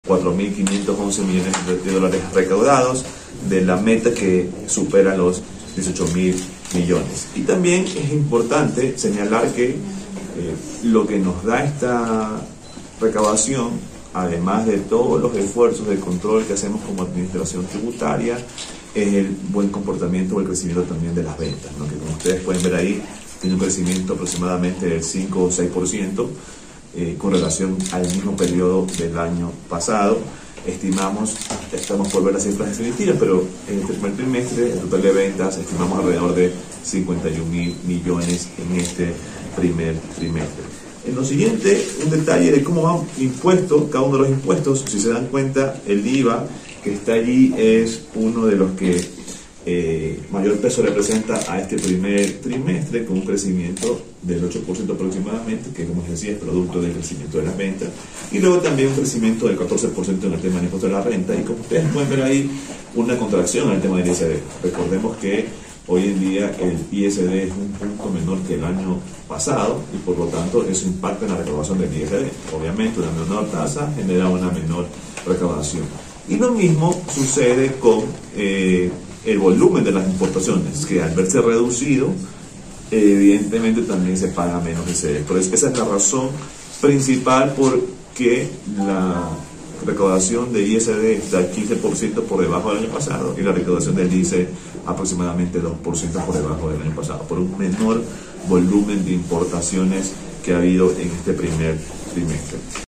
Más de 4 mil millones de dólares RECAUDADOS por el SRI 🇪🇨💵
Francisco Briones, Director General del SRI dió a conocer detalles de la recaudación de impuestos en el país.